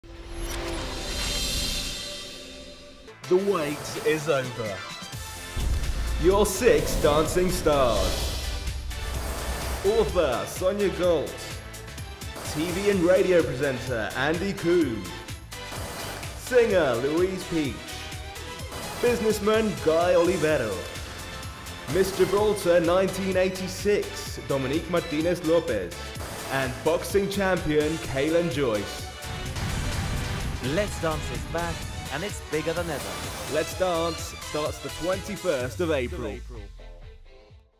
Let's Dance - The Contestants 2009
**Watch in HD by Clicking the HD button on the screen**

The Lets Dance 2009 Line-up

SONIA GOLT - Author & Bosom Buddies Organiser.
ANDY COUMBE - TV & Radio Presenter.
LOUISE PEACH - Singer.
GUY OLIVERO - Businessman.
DOMINIQUE MARTINEZ-LOPEZ - Miss Gibraltar 1986.
KAELAN JOYCE - Boxer.